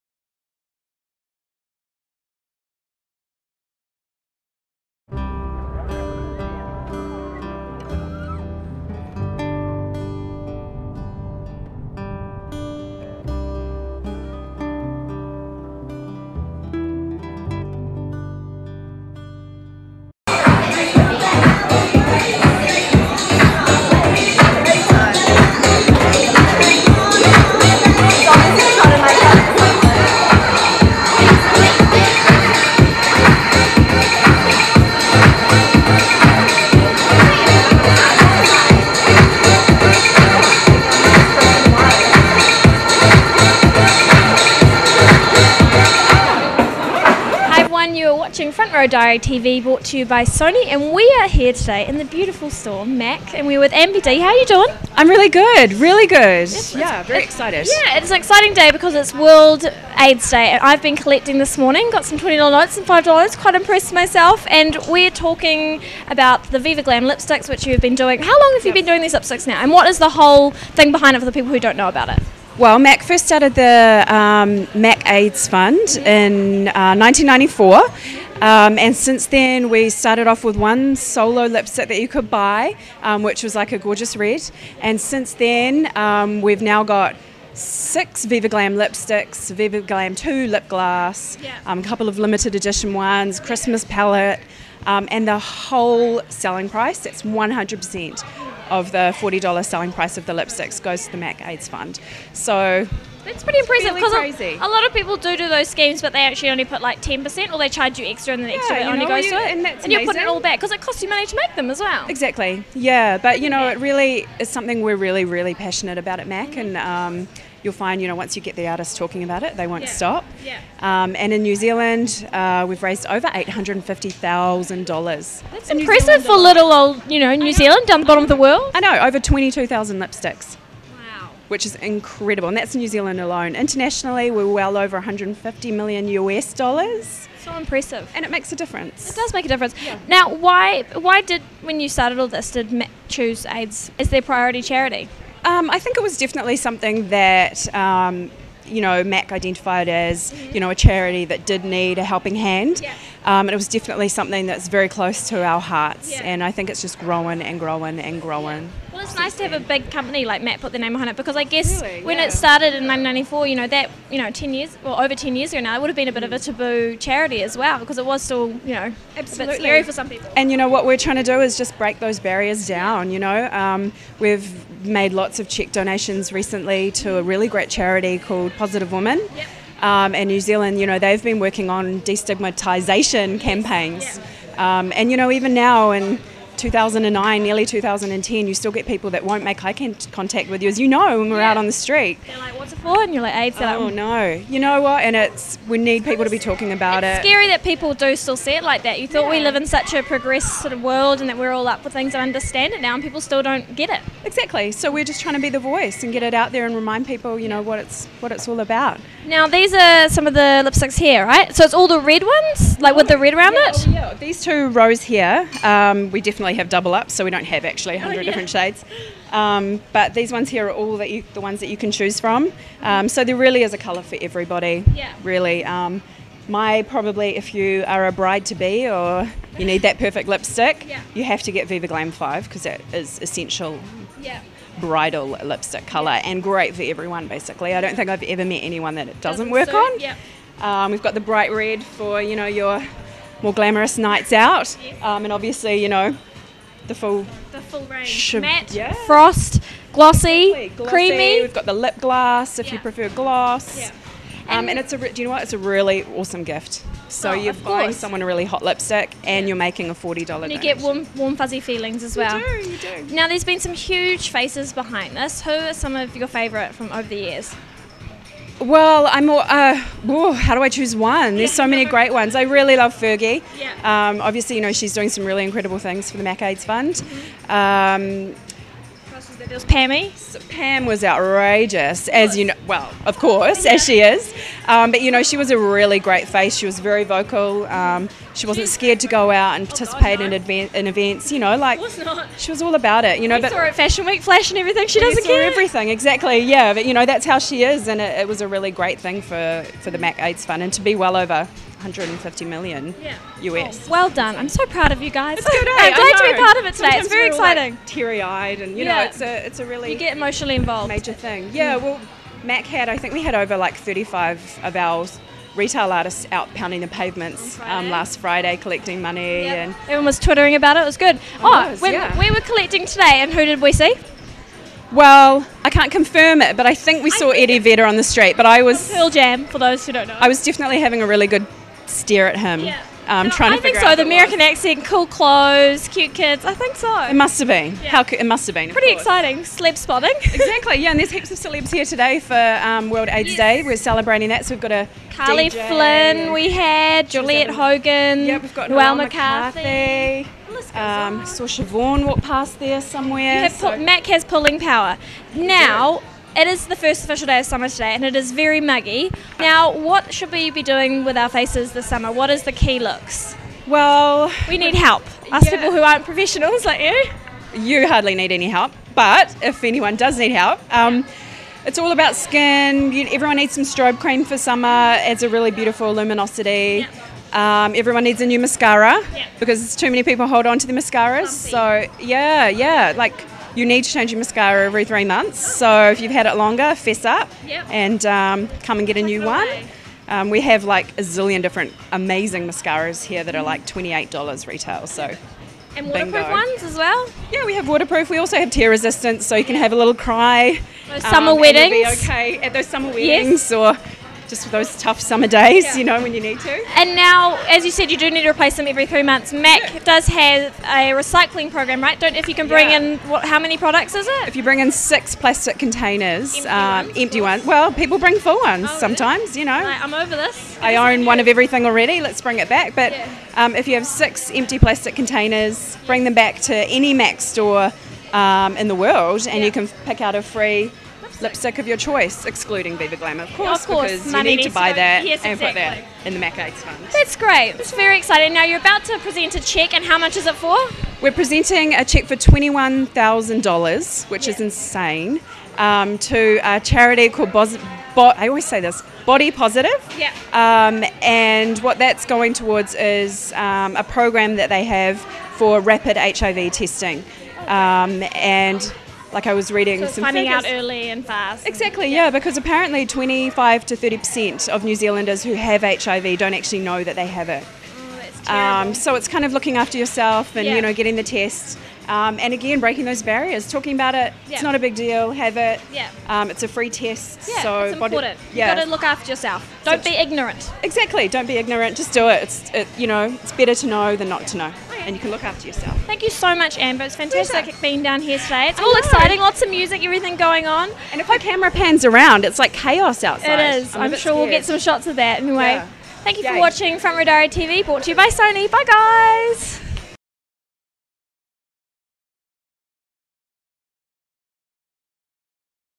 MAC chancery interview for World AIDS day
Anna was selling red ribbons and working at the MAC chancery store for World Aids Day! Join her with celebrities, music, buying a VIVA GLAM and make a difference!